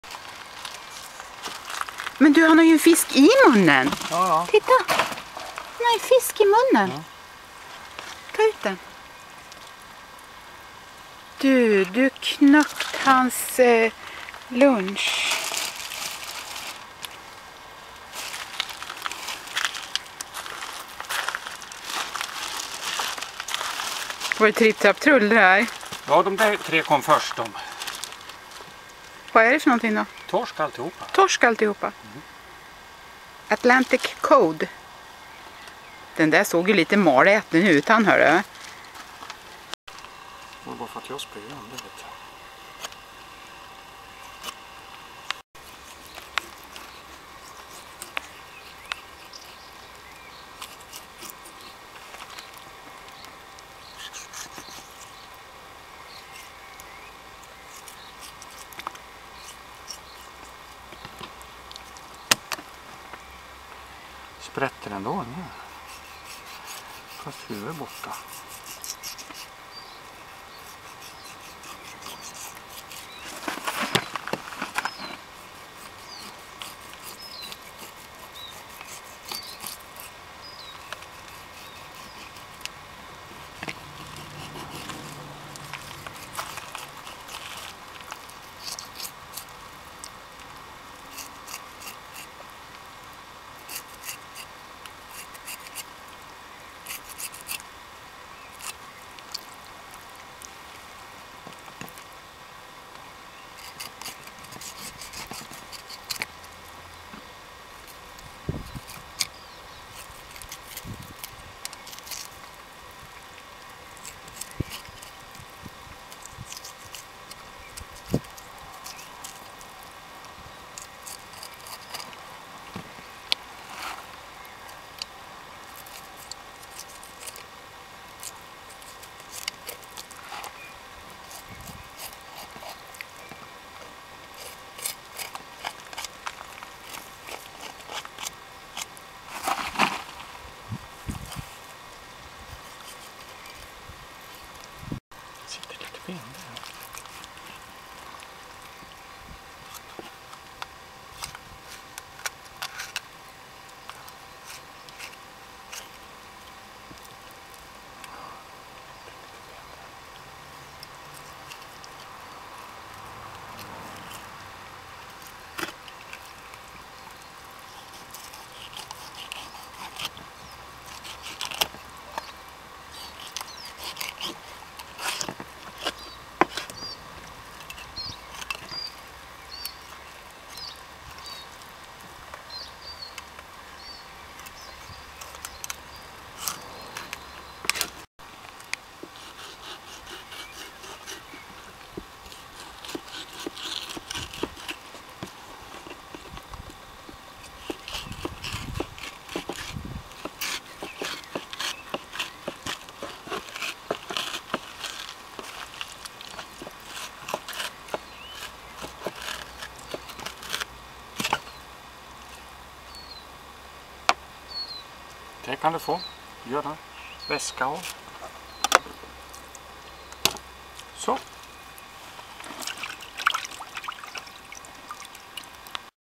Filéa torsk - skinna den
Atlantic Code - Catch it -  clean it  - cook it